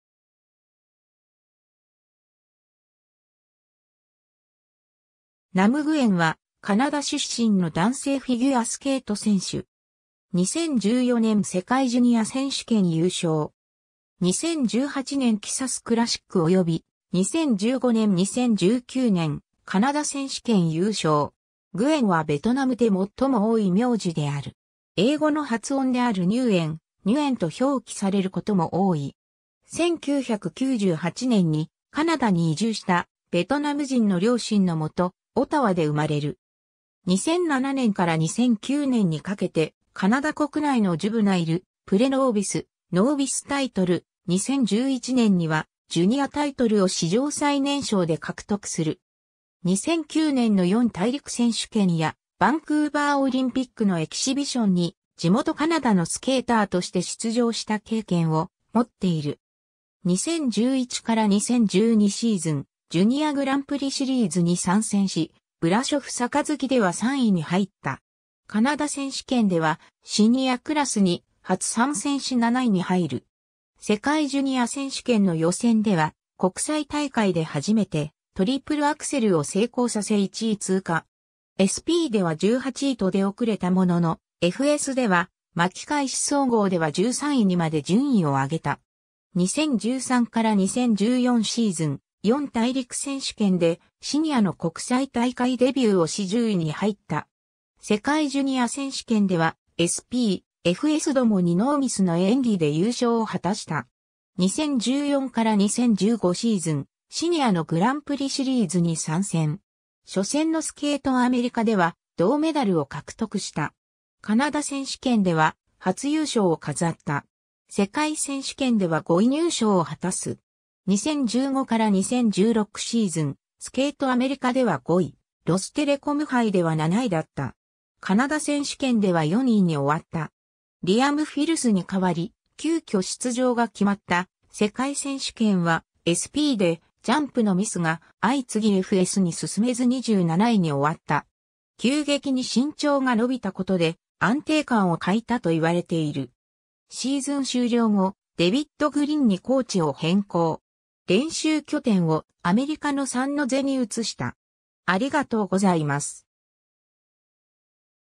ナム・グエン
ナム・グエン（チュハン：阮南、英語: Nam Nguyen、1998年5月20日 - ）は、カナダ出身の男性フィギュアスケート選手(男子シングル)。
 2014年世界ジュニア選手権優勝。
2018年CSUSクラシック及び、2015年・2019年カナダ選手権優勝。
 Nguyen(グエン)はベトナムで最も多い苗字である。
英語の発音であるニューエン、ニュエンと表記されることも多い。
 1998年にカナダに移住したベトナム人の両親のもと、オタワで生まれる。
2007年から2009年にかけて、カナダ国内のジュブナイル、プレノービス、ノービスタイトル、2011年にはジュニアタイトルを史上最年少で獲得する。
2009年の四大陸選手権やバンクーバーオリンピックのエキシビションに地元カナダのスケーターとして出場した経験を持っている。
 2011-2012シーズン、ジュニアグランプリシリーズに参戦し、ブラショフ杯では3位に入った。
カナダ選手権ではシニアクラスに初参戦し7位に入る。
世界ジュニア選手権の予選では国際大会で初めてトリプルアクセルを成功させ1位通過。
SPでは18位と出遅れたものの、FSでは巻き返し総合では13位にまで順位を上げた。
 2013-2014シーズン、四大陸選手権でシニアの国際大会デビューをし10位に入った。
世界ジュニア選手権ではSP、FS共にノーミスの演技で優勝を果たした。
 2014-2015シーズン、シニアのグランプリシリーズに参戦。
初戦のスケートアメリカでは銅メダルを獲得した。
カナダ選手権では初優勝を飾った。
世界選手権では5位入賞を果たす。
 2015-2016シーズン、スケートアメリカでは5位、ロステレコム杯では7位だった。
カナダ選手権では4位に終わった。
リアム・フィルスに代わり、急遽出場が決まった世界選手権は、SPでジャンプのミスが相次ぎFSに進めず27位に終わった。
急激に身長が伸びたことで安定感を欠いたといわれている。
シーズン終了後、デヴィッド・グリンにコーチを変更。
練習拠点をアメリカのサンノゼに移した。